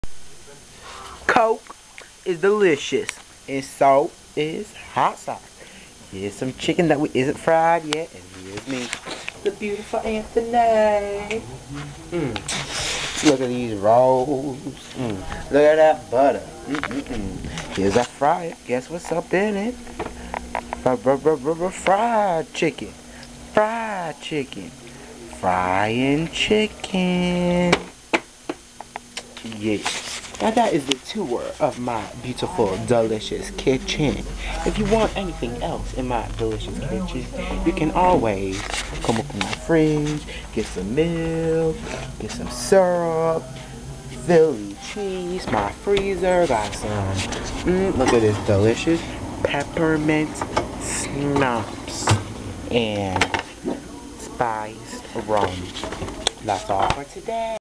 Kitchen Tour
Im up in my kitchen frying chicken so i decided ta make this clip. prty random